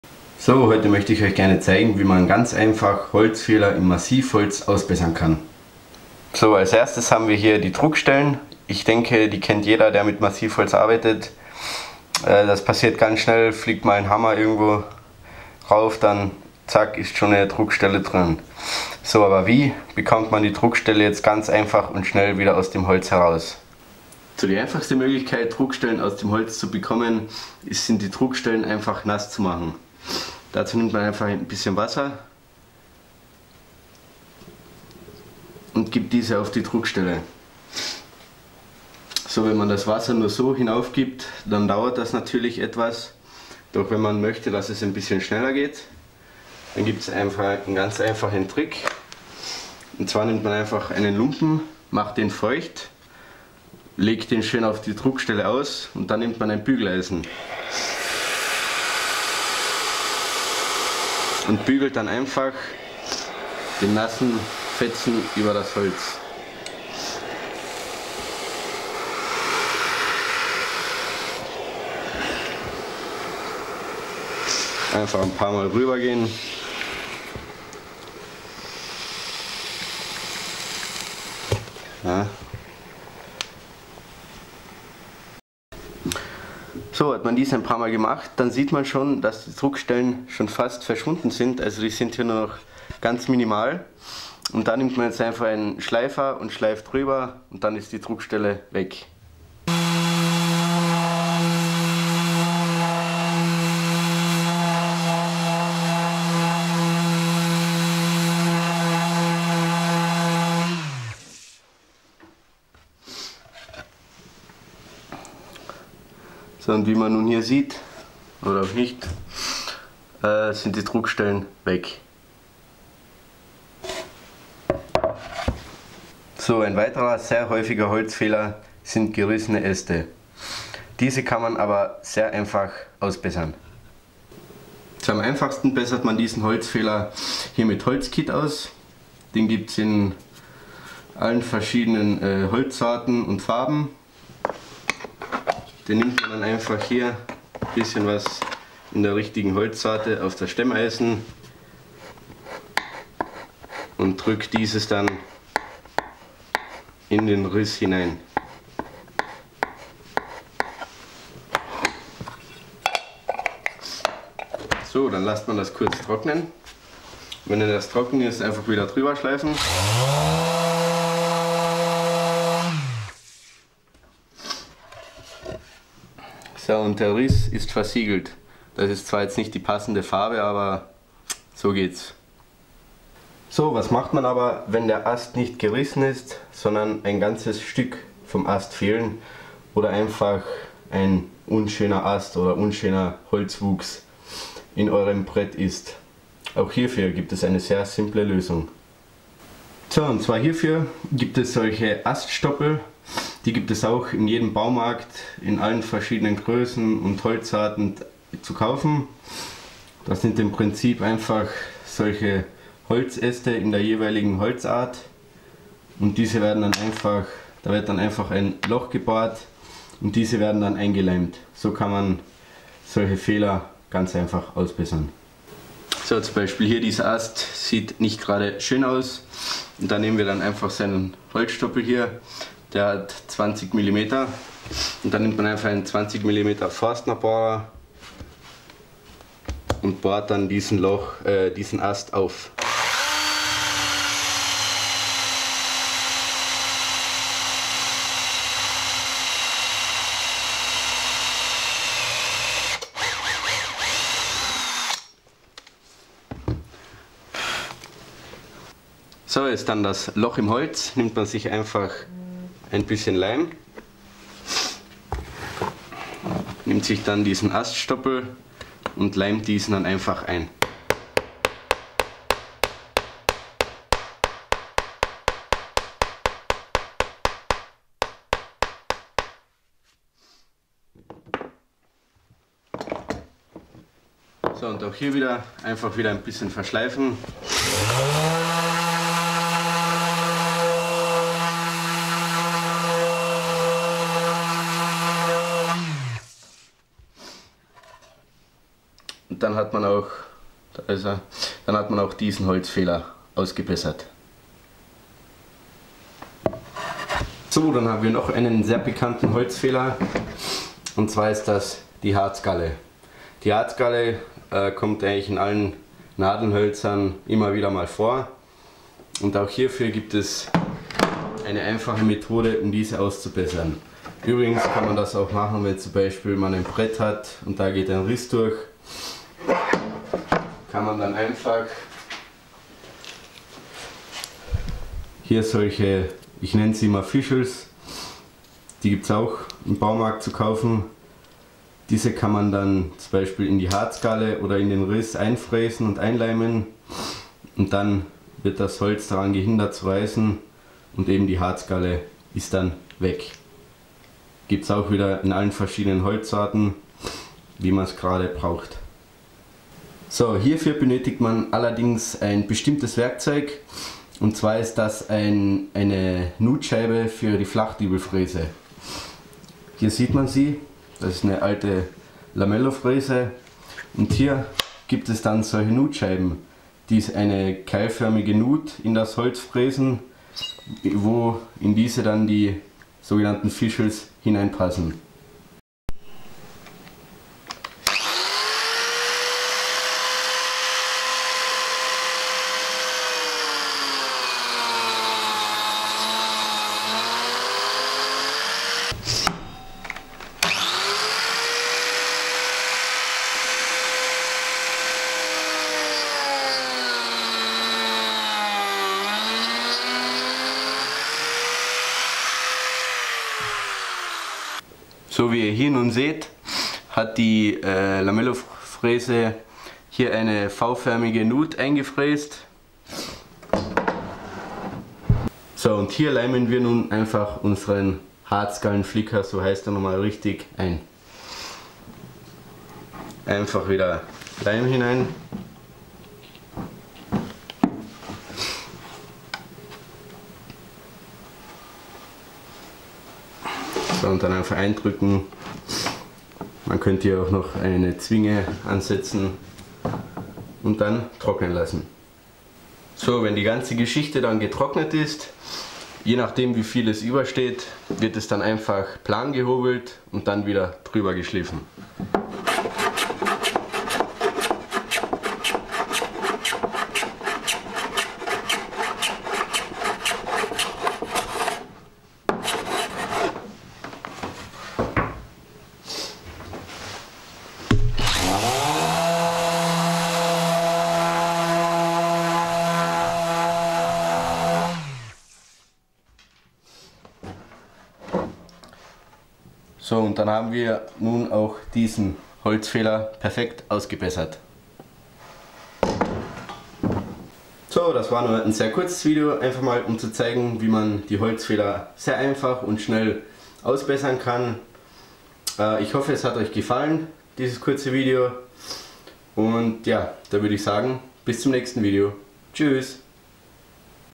Holzfehler in Massivholz ausbessern
Hallo heute zeige ich euch wie ihr ganz einfach und schnell die verschiedensten Holzfehler  ausbessern könnt. Viel Spaß dabei

 Maschinen:
*Lamello Fräse
Mein Werkzeug:
*Längsschnitt Sägeblatt
*das beste Holzfinish

Wenn du die Artikel über meinen Link kaufst bekomme ich davon eine kleine Provision. Du musst nicht mehr bezahlen.